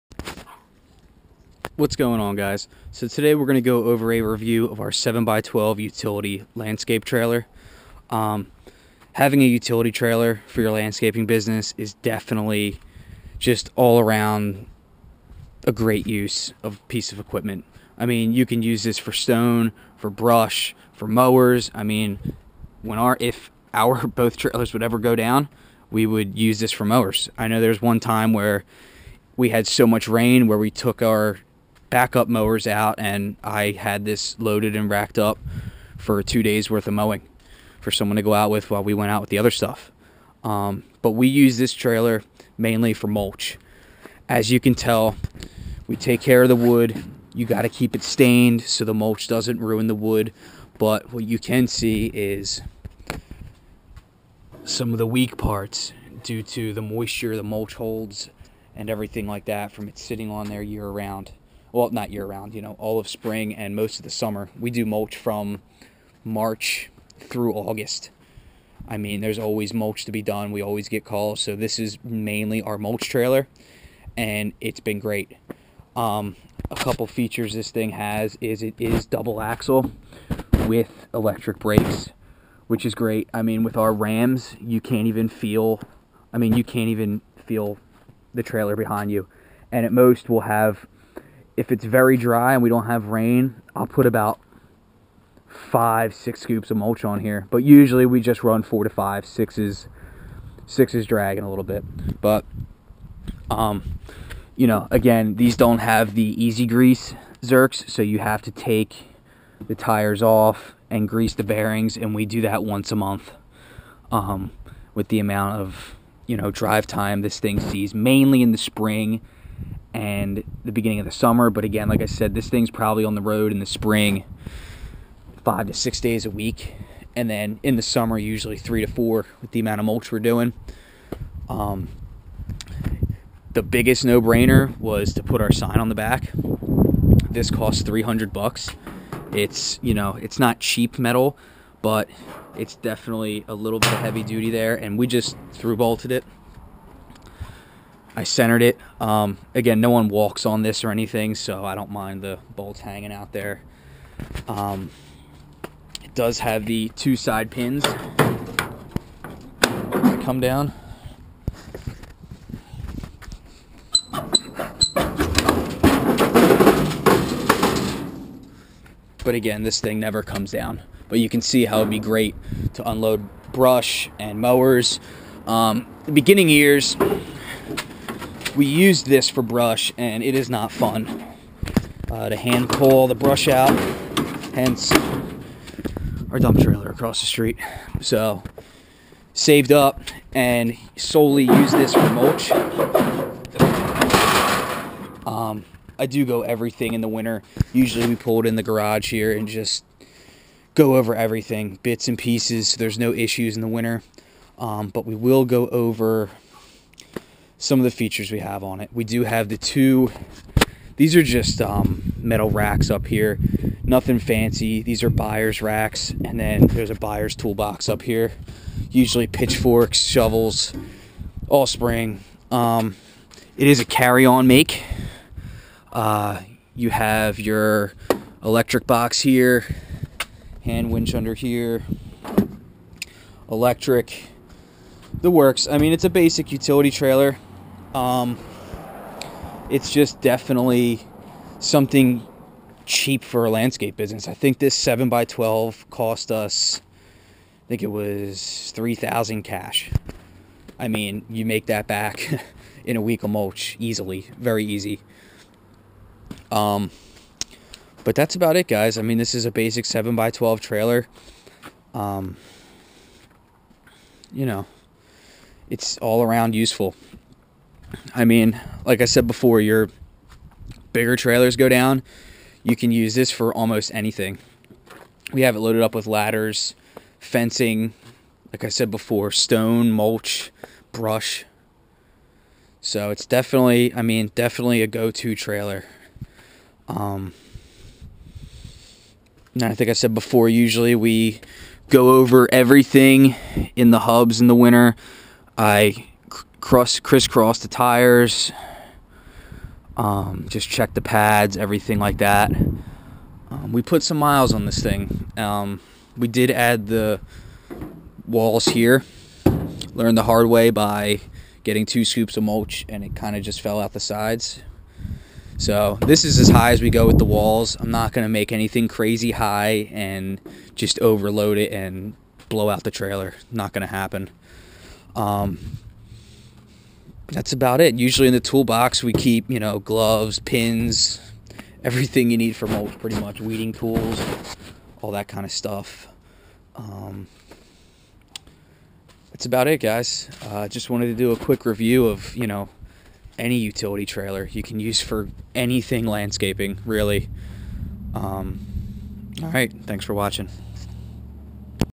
7X12 Carry On Utility Landscaping Trailer Review
Today we go over our three year review of our 7x12 utility landscaping mulch trailer!

A Frame Tounge
Payload Capacity 7,200
Double Axle
Electric Brakes
Assist Spring Ramp Gate
Make: Carry On Trailers